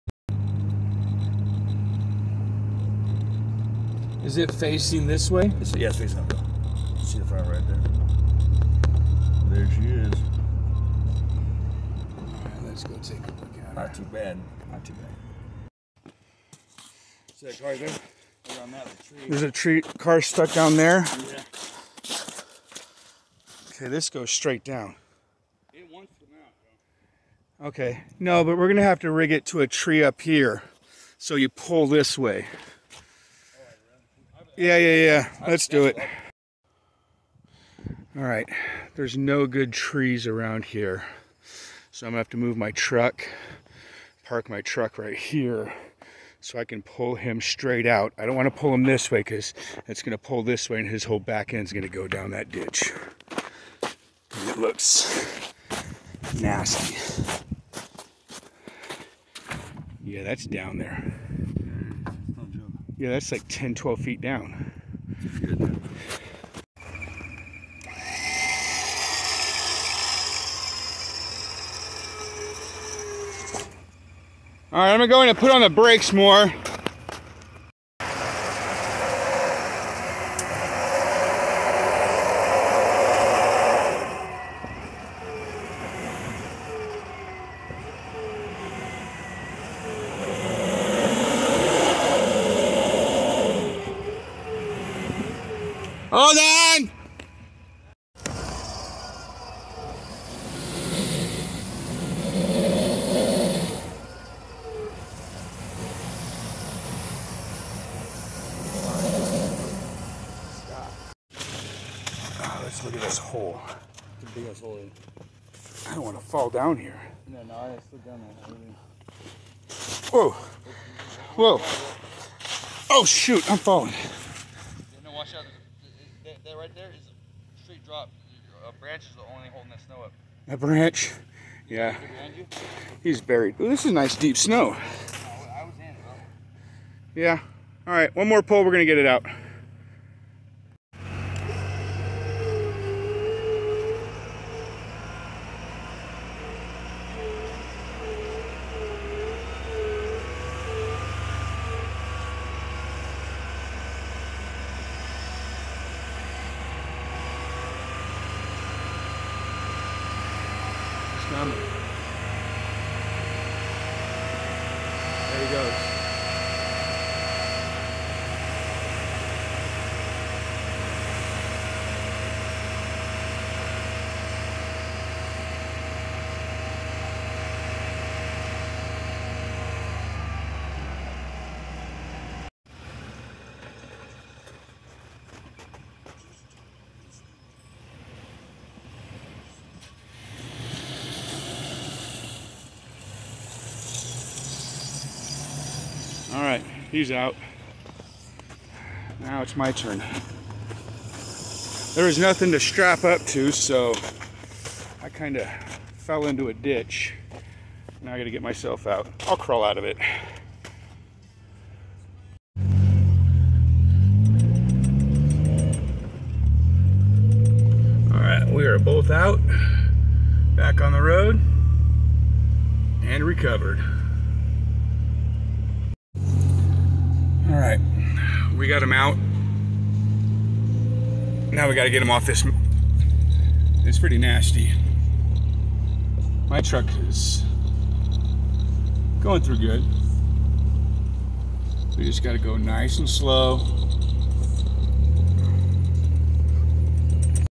Toyota Tacoma snow recovery
3rd gen Toyota Tacoma almost slides of mountain in the snow.  I use my winch to recover the Tacoma